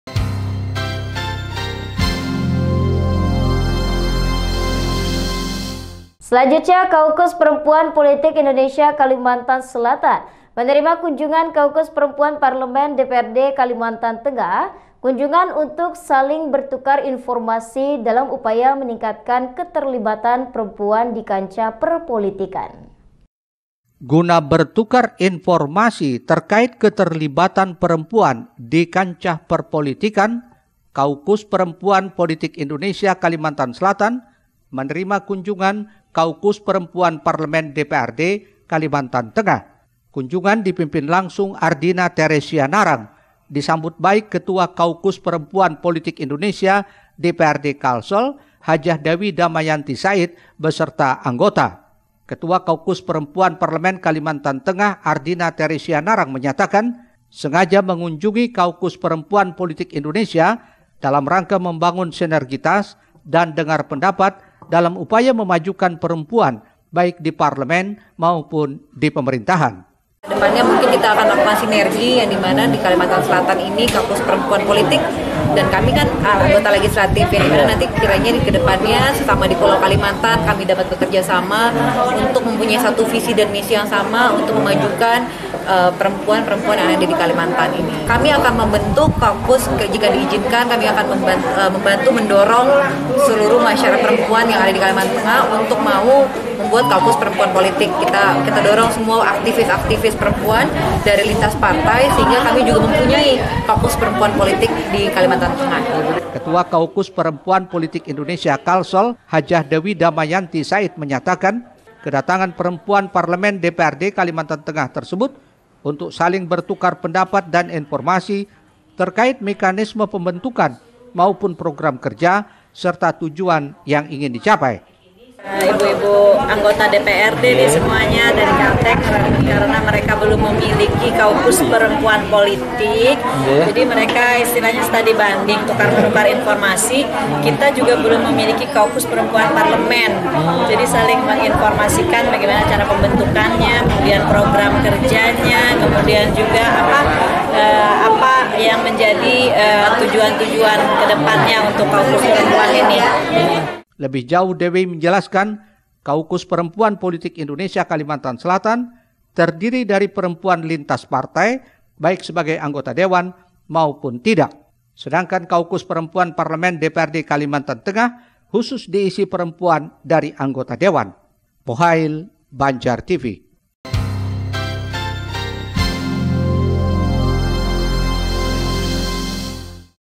SALING TUKAR INFORMASI LIBATKAN PEREMPUAN DIKANCAH POLITIK
Kaukus Perempuan Politik Indonesia Kalimantan Selatan menerima kunjungan Kaukus Perempuan Parlemen DPRD Kalimantan Tengah. Kunjungan untuk saling bertukar informasi, dalam upaya meningkatkan keterlibatan perempuan di kancah perpolitikan.